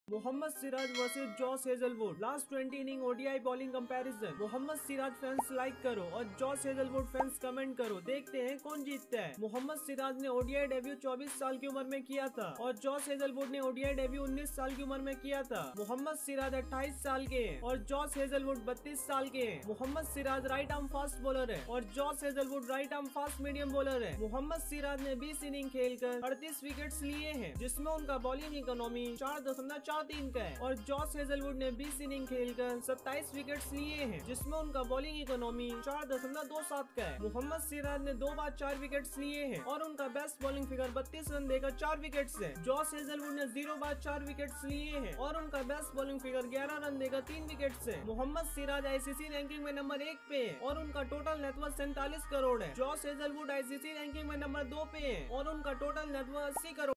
Mohammed Siraj vs Josh Hazlewood Bowling Comparison || 137
Mohammed Siraj vs Josh Hazlewood ODI Bowling Comparison

Comparison
ODI Bowling Comparison
Name
Debut
Age
Bowling Style
Innings
Wickets
Bowling Economy
Four Wickets
Best Bowling Figure
ICC Ranking
Net Worth
Winner